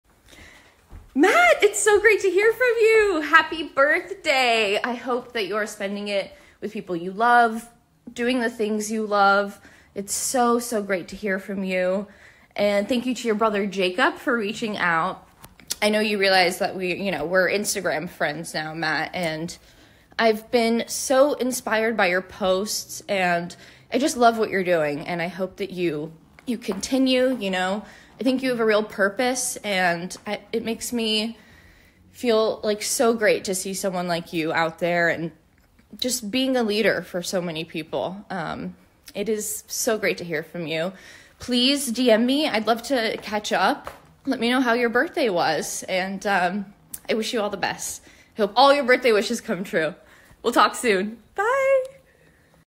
Birthday Message from Medalion Rahimi
I got a Cameo message and a nice birthday message from Medallion Rahimi who starred as  Fatima Namazi in NCIS Los Angeles